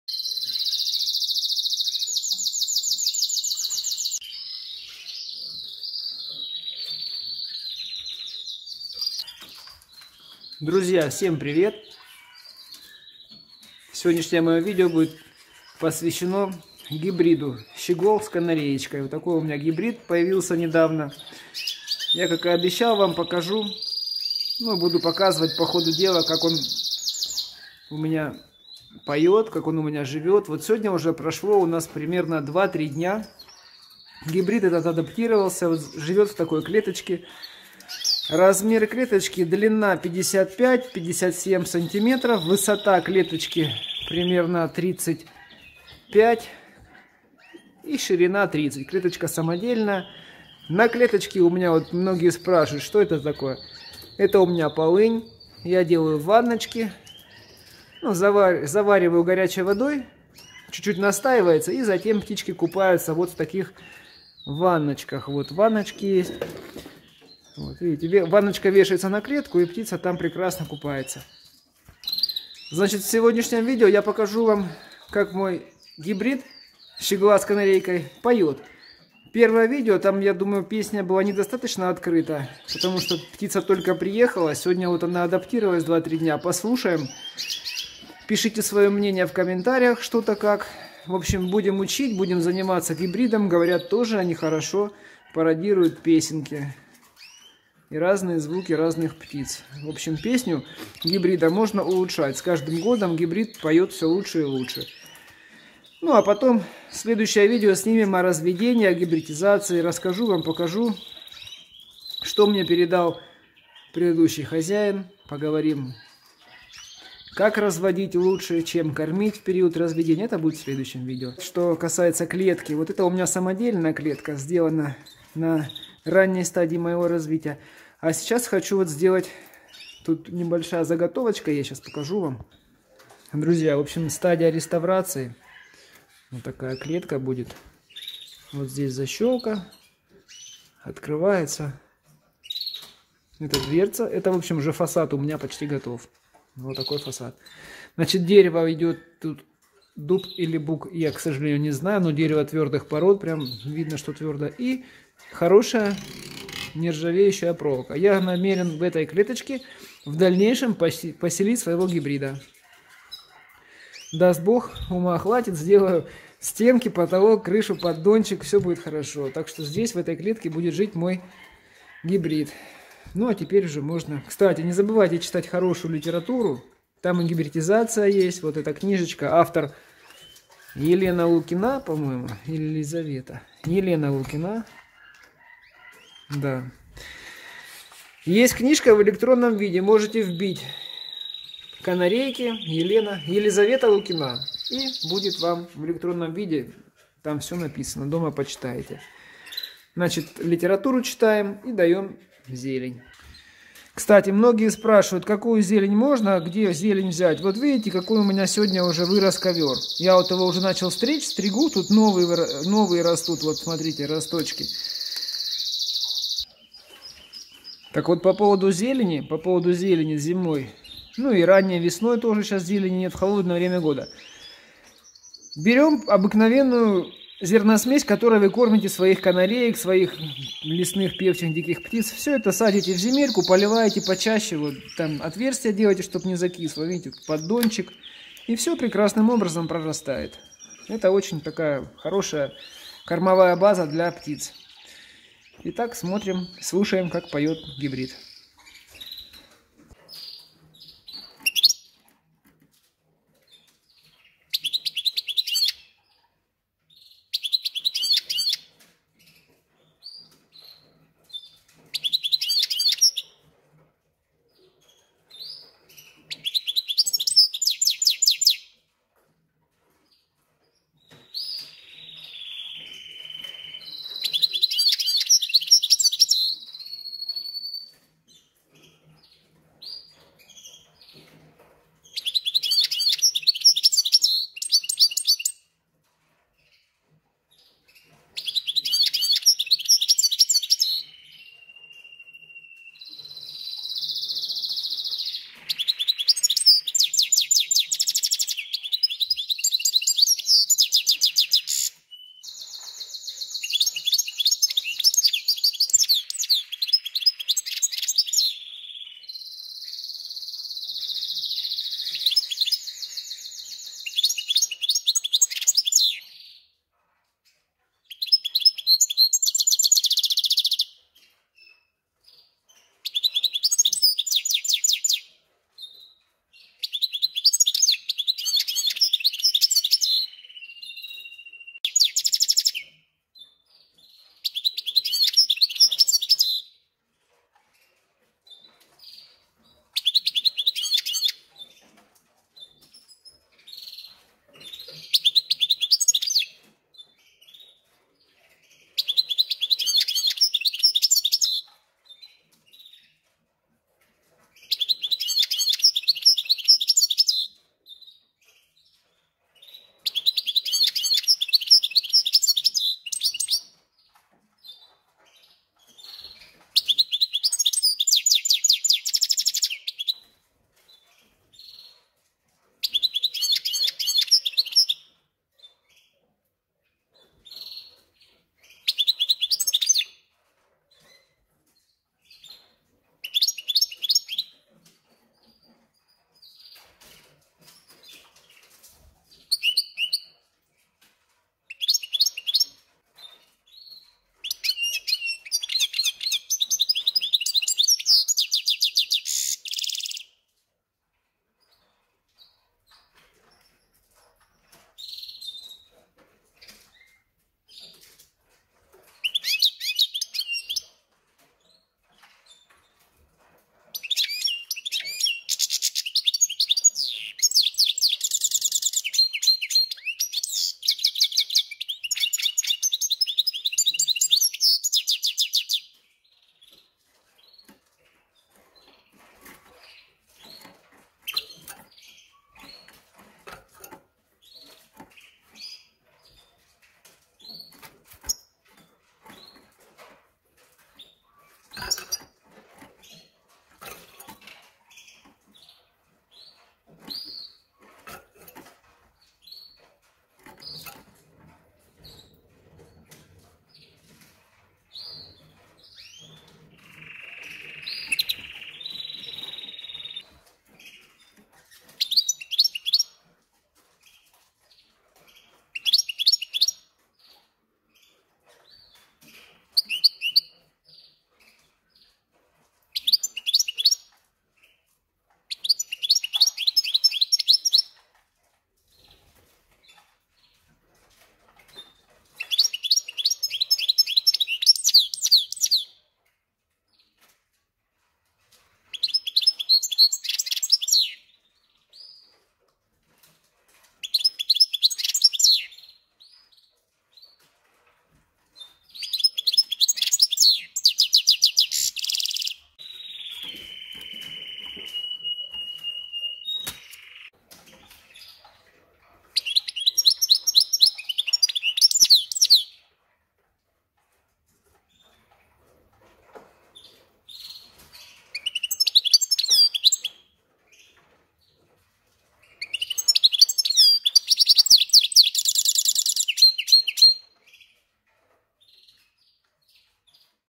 Поющий Гибрид Щегла и Канарейки,Содержание гибрида щегла,
В этом видео гибрид поёт ,гибрид щегла и канарейки выходит на песню.Содержание гибрида щегла и канарейки и пение гибрида бывает разным ,гибрид так же как канарейка может копировать пенье птиц.Всё начиналось с разведение канареек ! и вот теперь у нас появился Поющий Гибрид Щегла и Канарейки !